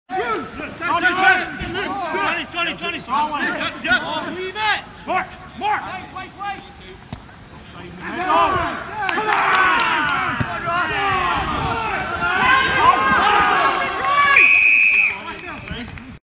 Close, but no cigar
It was an 8 try thriller at Kilmacolm between Birkmyre and Waysiders/Drumpellier in the West Bowl - here's one that just got away.